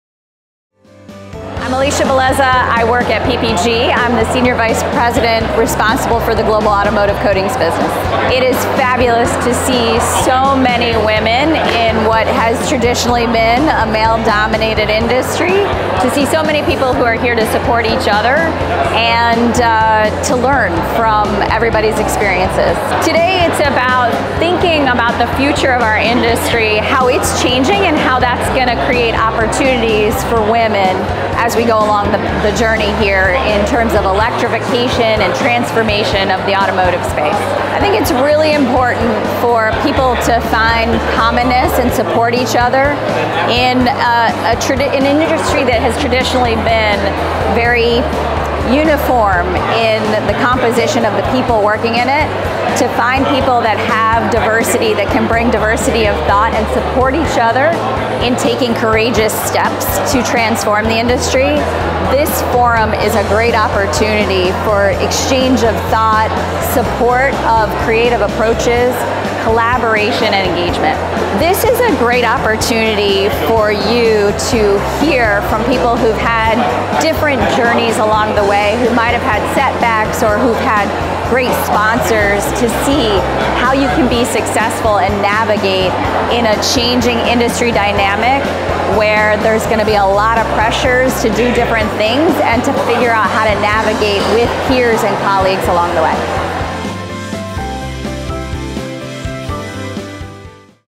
Interview: Alisha Bellezza, SVP, Global Automotive Coatings, PPG
Women Automotive Summit: Detroit : 4th June 2024, Suburban Collection, Novi, MI